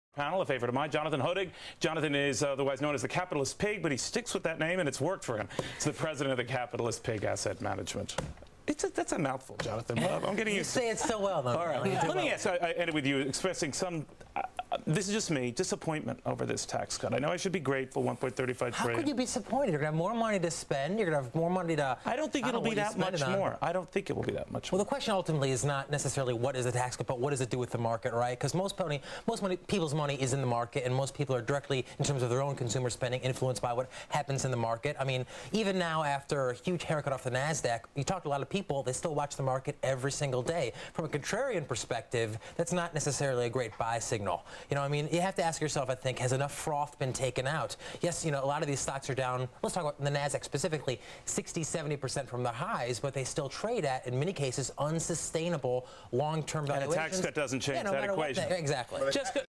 Jonathan Hoenig with Neil Cavuto on Bush tax cuts, June 2001
Jonathan Hoenig, portfolio manager at Capitalistpig Hedge Fund LLC discusses impact of the Bush tax cuts on the stock market with Neil Cavuto on Fox News Channel in June 2001.